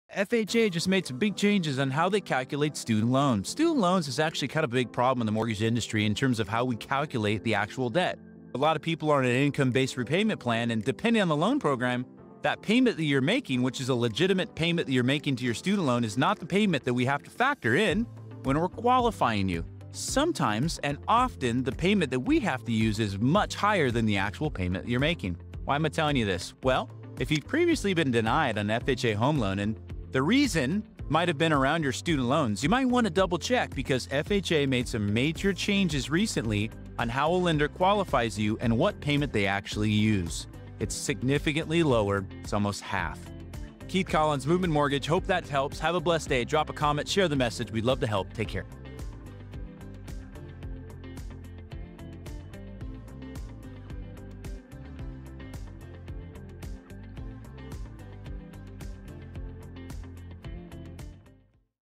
Denied for a Home Loan Because of Student Loans?
FHA just made some significant changes in how they calculate student loans.

Student loans are a big problem in the mortgage industry in terms of calculating the actual debt.

Sometimes and often, the payment that we have to use is much higher than the actual payment you're making.

Why am I telling you this?

Well, if you've previously been denied an FHA home loan, and the reason might have been around your student loans. You might want to double-check because FHA made some significant changes recently on how a lender qualifies you and what payment they use.

It's significantly lower. It's half.

Keith Collins - Mortgage Guy